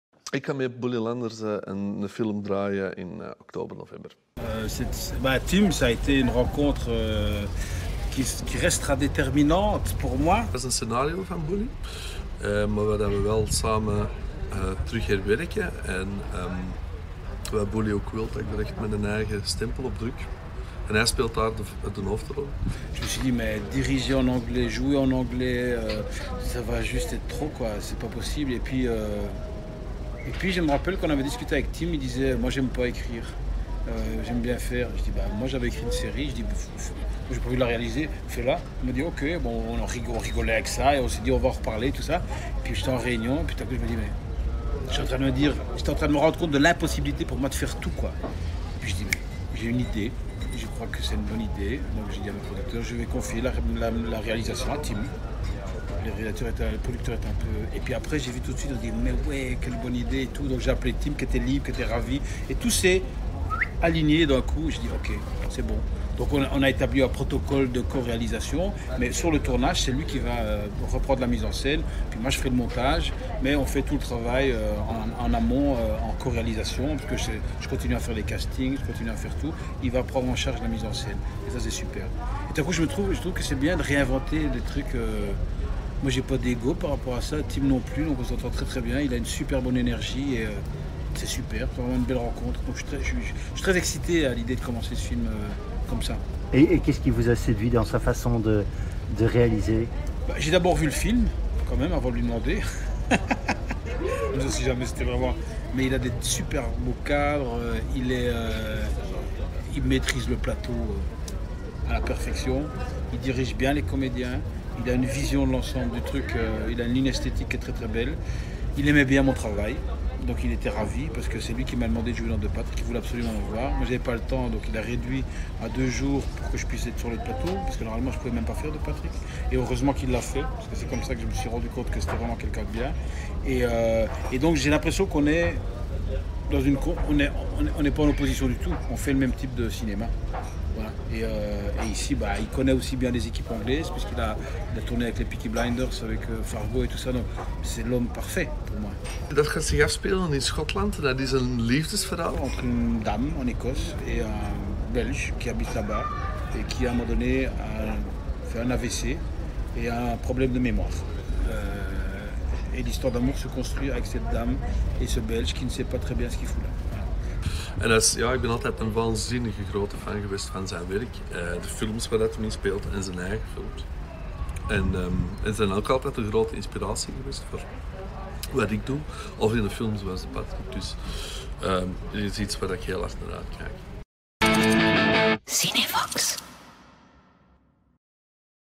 Tim Mielants & Bouli Lanners over hun toekomstige samenwerking
Tim Mielants gaat 'Wise Blood', de nieuwe film van Bouli Lanners regisseren!